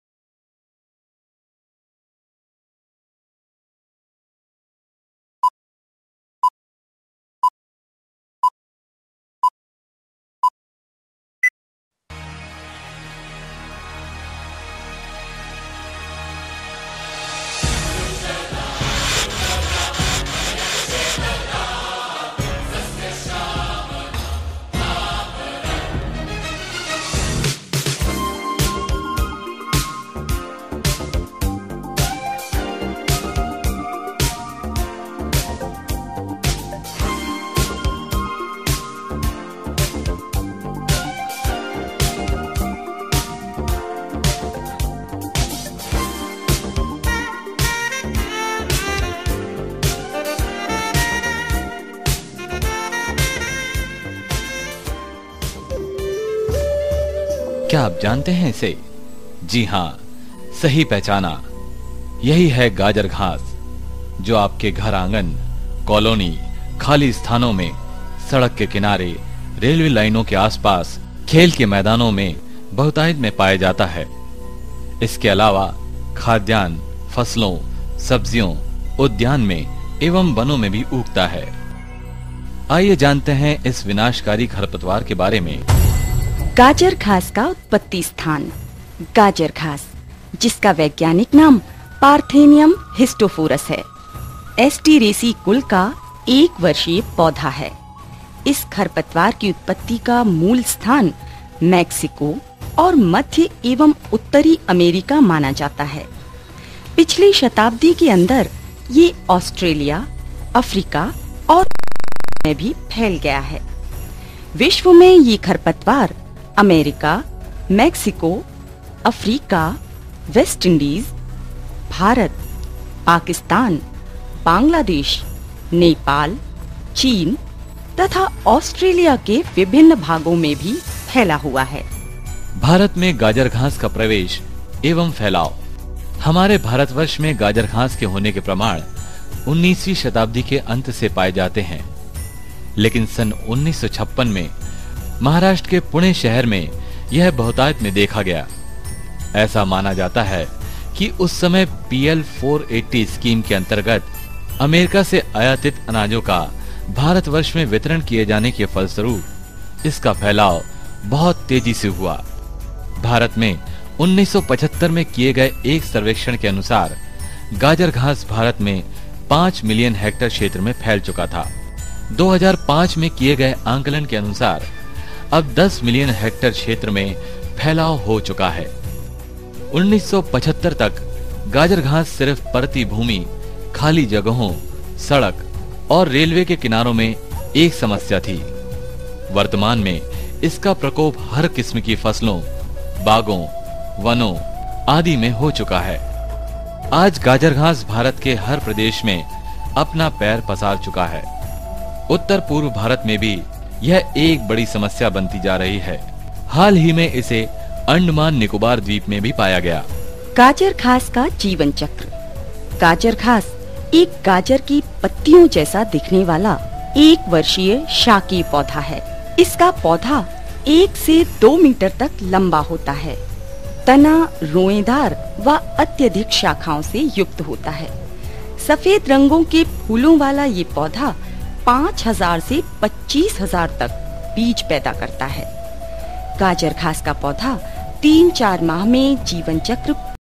Parthenium Video Film
Present by Director, Dr Jay G. Varshney and Dr. Sushilkumar, Sr. Scientist, Entomology, Biocontrol of Parthenium to Mexican beetle, "Zygogramma bicolorata" .
Gyanendra Singh